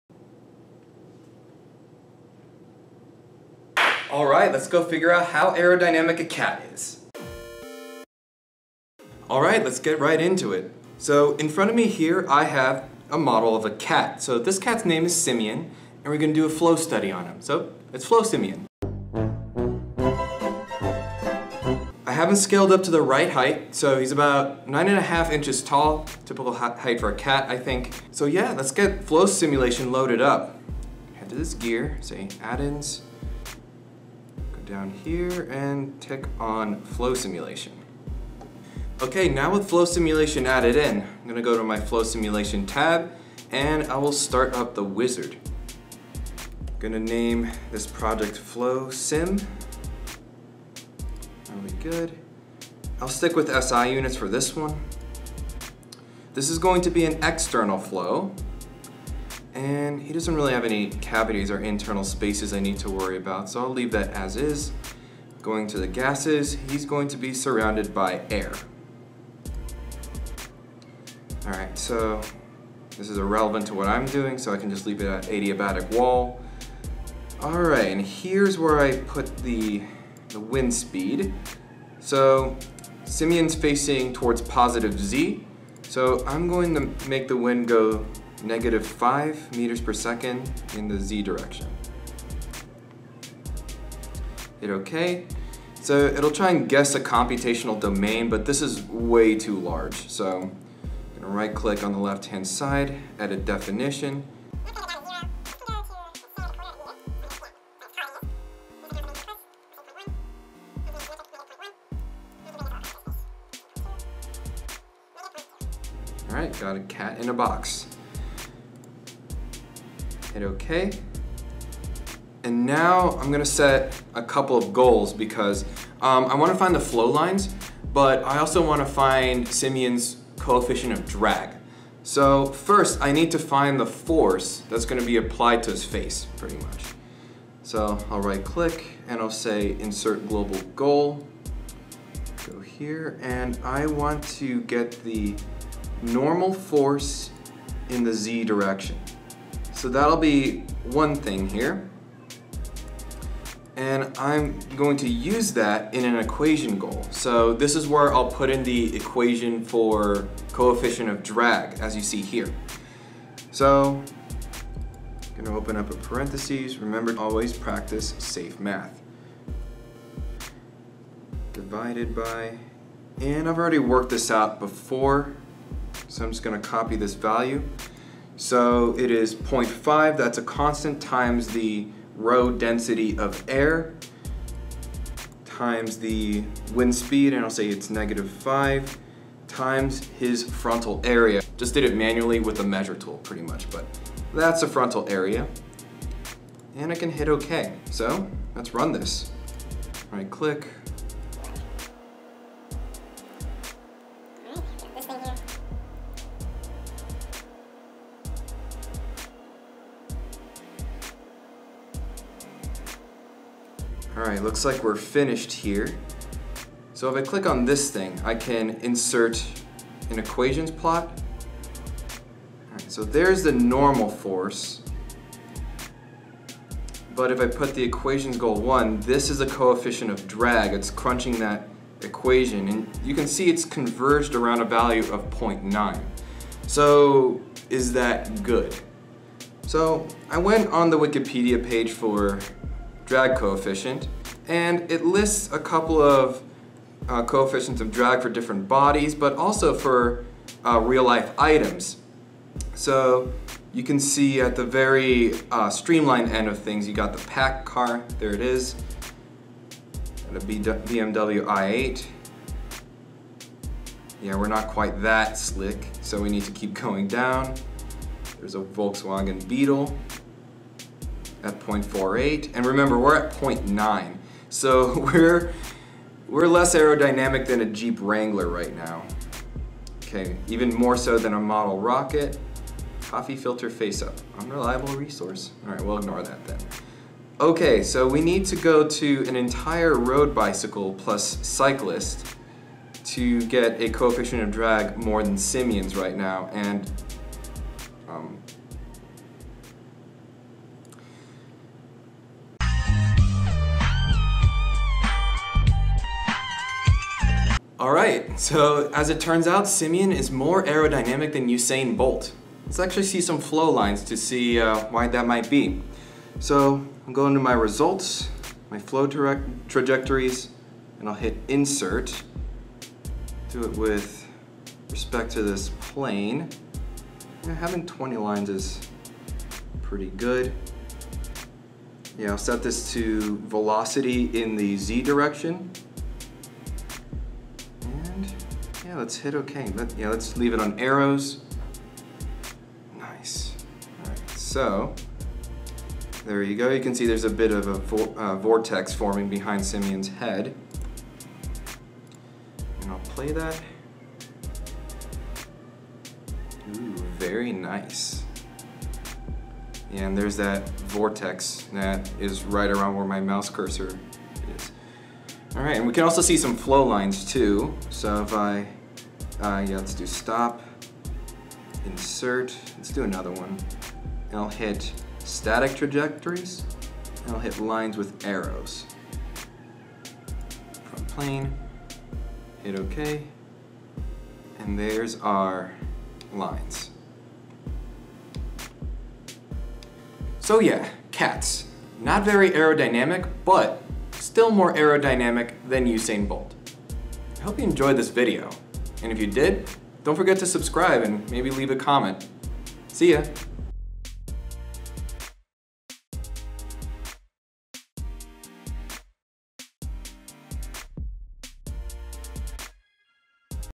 AERODYNAMICS OF CAT (Tutorial)
Hey guys!

This time I hop back on the meme bandwagon again and show you how to compute how aerodynamic a cat is. I'm not sorry.